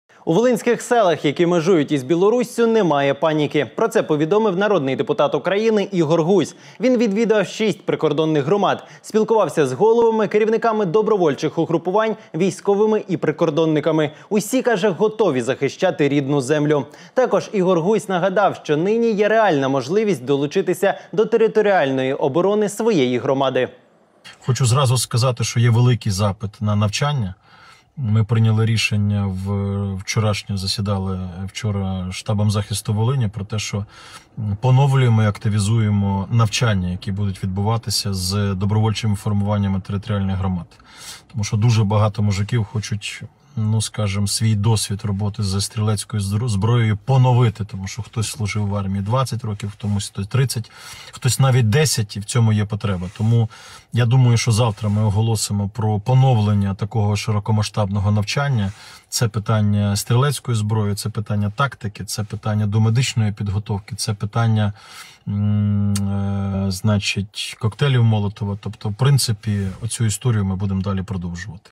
На Волині організують навчання для добровольчих формувань громад, – нардеп
Народний депутат Ігор Гузь нагадав, що нині є реальна можливість долучитися до територіальної оборони своєї громади.

«Ми вчора засідали штабом захисту Волині і прийняли рішення про те, що поновлюємо і активізуємо навчання, які будуть відбуватися з добровольчими формуваннями територіальних громад», – заявив він.